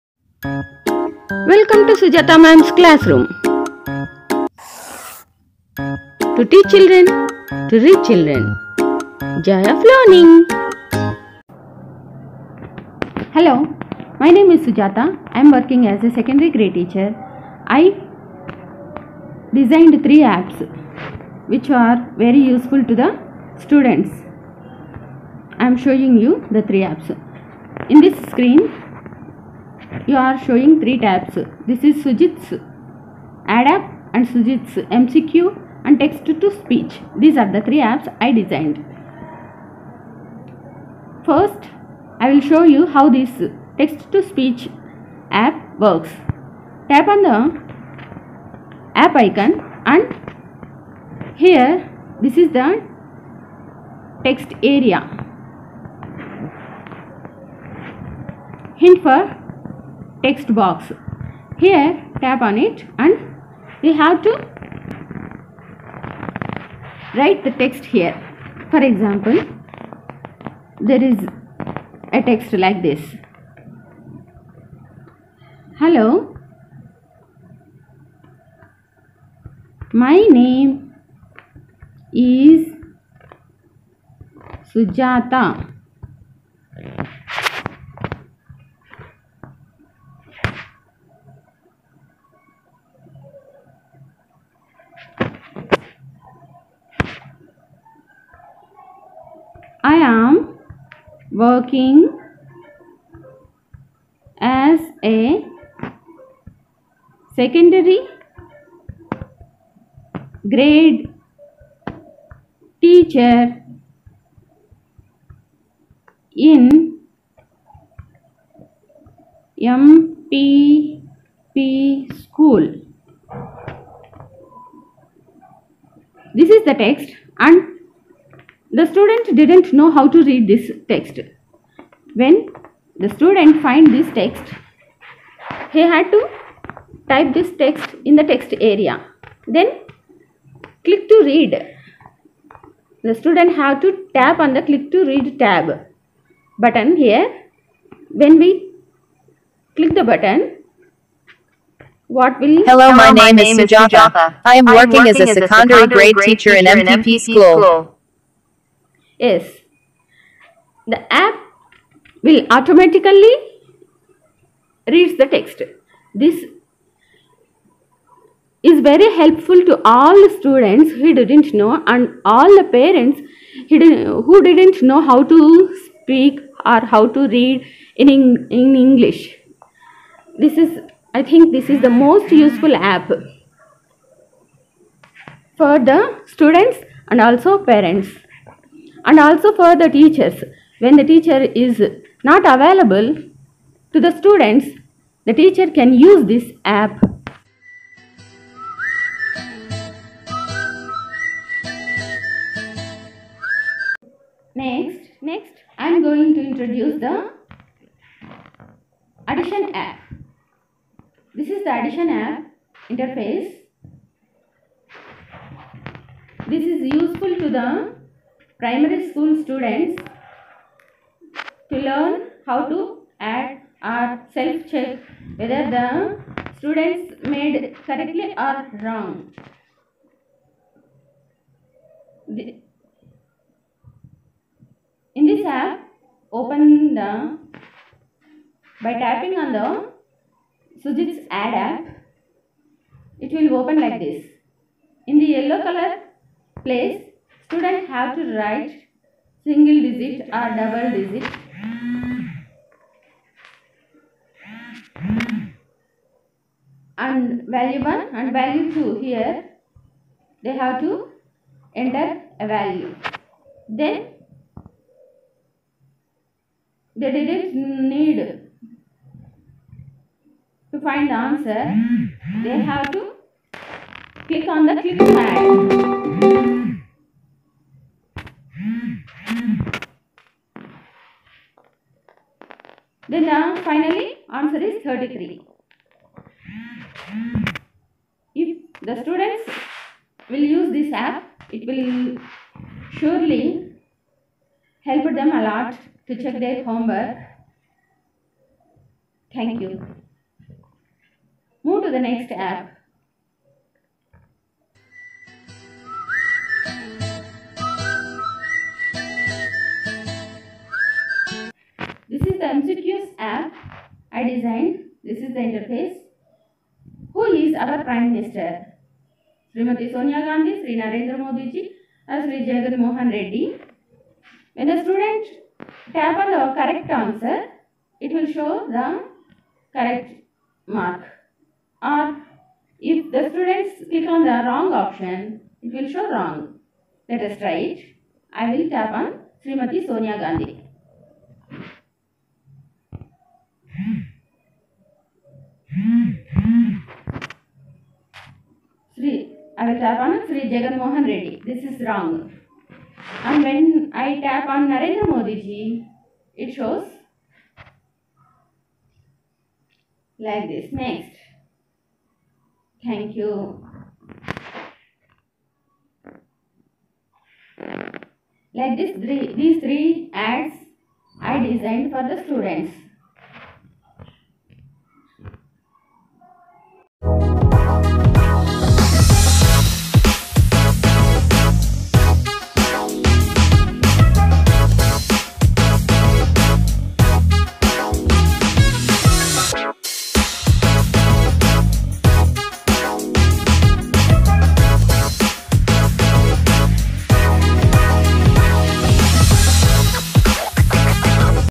Sujith's apps sujathathudimella Apps educationalvideosforkids apps#onlineclasses
ఆంగ్లం చదవడం లో ఇబ్బంది పడుతున్నారా? అయితే ఈ వీడియో మీకోసమే.
గణితం లో కూడికలు చేశాక ఒక సారి ఈ ఆప్ లో చెక్ చేసుకోండి పిల్లలూ.

మీరు కనుక ఈ ఆప్స్ డౌన్లోడ్ చేసుకోవాలనుకుంటే క్రింద ఇచ్చిన లింక్ ద్వారా నా టెలిగ్రాం గ్రూప్ లో చేరండి.
I designed text to speech app plz share with your students

Useful for reading practice period

To discuss more on my videos join my telegram group

I designed 3 educational apps

They will assist my students to read English
To check additions
To evaluate a teacher can use MCQ app

Please watch and express ur views in the form of comments in my channels comments box.

The most useful app Text to speech

useful for all students who don't know how to read the English text book Or to learn questions and answers
must down load from the link and use it

ఇంగ్లీష్ భాష చదవడం కానివారికి ఉపయోగపడే చక్కని ఆప్.
ఇది ఆఫ్ లైన్లో కూడా పనిచేస్తుంది.